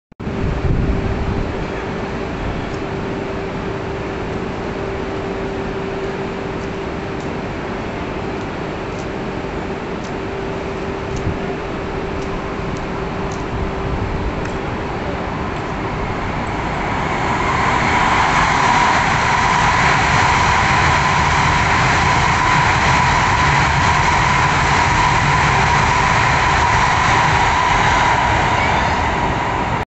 東北新幹線 福島通過
2022/5/2撮影。
同3/16に発生した地震の影響により、減速運転による臨時ダイヤでの運転でした。
14番線に停車中の上りやまびこ188号は仙台→福島では100キロ前後、福島→郡山では200キロ前後での運転だったと記憶しています。